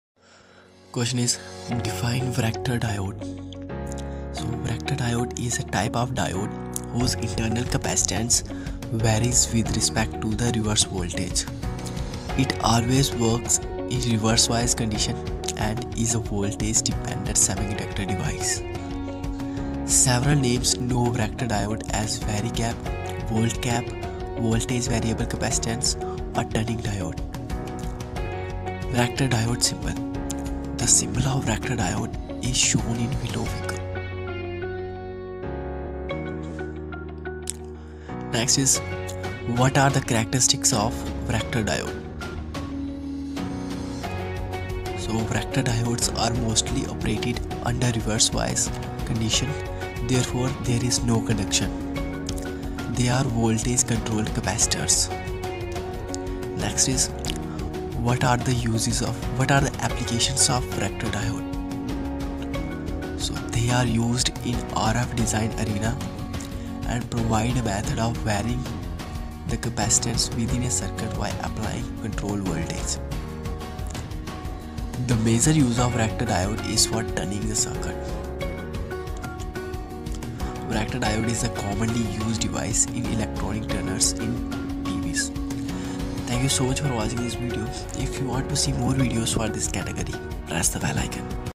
Q.Define Varactor diode ? Symbol, characteristics and what are the application of varactor diode ?
Ans.Varactor diode is a type of diode whose internal capacitance varies with respect to the reverse voltage. It always works in reverse bias conditions and is a voltage-dependent semiconductor device. Several names know varactor diode as Varicap, Voltcap, Voltage variable capacitance, or Tunning diode.

characteristics of varactor diode

Varactor diodes are mostly operated under reverse bias conditions; therefore, there is no conduction. They are voltage-controlled capacitors and are sometimes known as varicap diodes, although the word varactor is widely used.

Applications of varactor diode

1.They are used in the RF design arena and provide a method of varying the capacitance within a circuit by applying control voltage.
2.The major use of the varactor diode is for tuning the circuit.
3.Varactor diode is the commonly used diode in electronic tuners in TVs.